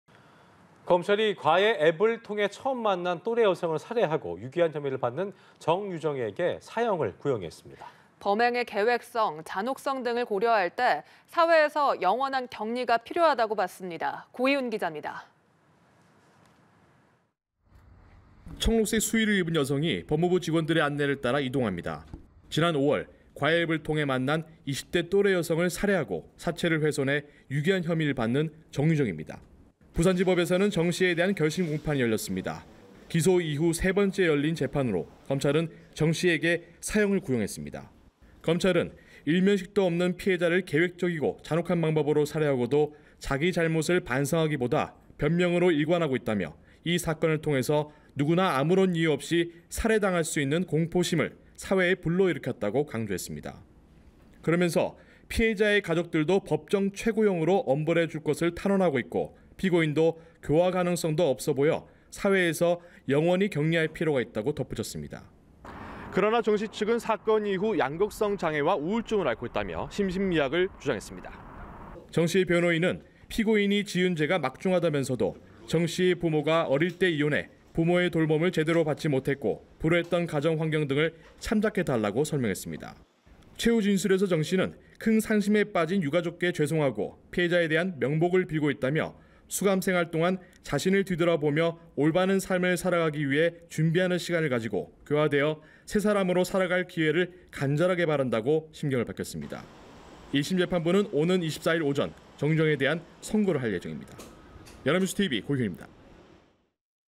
'과외앱 살해' 정유정에 검찰, 사형 구형…"영원한 격리 필요" / 연합뉴스TV (YonhapnewsTV)
'과외앱 살해' 정유정에 검찰, 사형 구형…"영원한 격리 필요"

[앵커]

검찰이 과외앱을 통해 처음 만난 또래 여성을 살해하고 유기한 혐의를 받는 정유정에게 사형을 구형했습니다.

범행의 계획성, 잔혹성 등을 고려할 때 사회에서 영원한 격리가 필요하다고 봤습니다.

고휘훈 기자입니다.

[기자]

청록색 수의를 입은 여성이 법무부 직원들의 안내를 따라 이동합니다.

지난 5월, 과외앱을 통해 만난 20대 또래 여성을 살해하고 사체를 훼손해 유기한 혐의를 받는 정유정입니다.

부산지법에서는 정 씨에 대한 결심공판이 열렸습니다.

기소 이후 열린 3번째 재판으로, 검찰은 정 씨에게 사형을 구형했습니다.

검찰(부산지검 형사3부)은 "일면식도 없는 피해자를 계획적이고 잔혹한 방법으로 살해하고도 자기 잘못을 반성하기보다 변명으로 일관하고 있다"며 "이 사건을 통해서 누구나 아무런 이유 없이 살해당할 수 있는 공포심을 사회에 불러일으켰다"고 강조했습니다.

그러면서 "피해자의 가족들도 법정최고형으로 엄벌해줄 것을 탄원하고 있고, 피고인도 교화 가능성도 없어 보여 사회에서 영원히 격리할 필요가 있다"고 덧붙였습니다.

그러나 정 씨 측은 사건 이후 양극성 장애와 우울증을 앓고 있다며 심신미약을 주장했습니다.

정 씨의 변호인은 "피고인이 지은 죄가 막중하다"면서도 "정 씨의 부모가 어릴 때 이혼해 부모의 돌봄을 제대로 받지 못했고, 불우했던 가정 환경 등을 참작해 달라"고 설명했습니다.

최후 진술에서 정 씨는 "큰 상심에 빠진 유가족께 죄송하고 피해자에 대한 명복을 빌고 있다"며 "수감생활 동안 자신을 뒤돌아보며 올바른 삶을 살아가기 위해 준비하는 시간을 가지고, 교화되어 새 사람으로 살아갈 기회를 간절하게 바란다"고 심경을 밝혔습니다.

1심 재판부는 오는 24일 오전 정유정에 대해 선고할 예정입니다.

연합뉴스TV 고휘훈입니다.